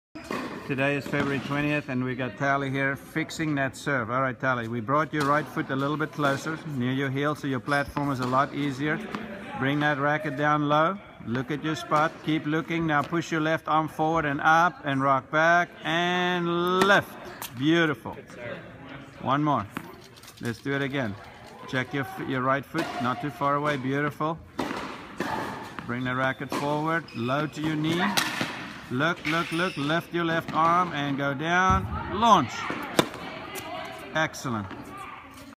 Tali Jarmond new serve - Johan Kriek February 2015
Tali Jarmond, 10 years old - new serve - Johan Kriek Tennis Academy - February 2015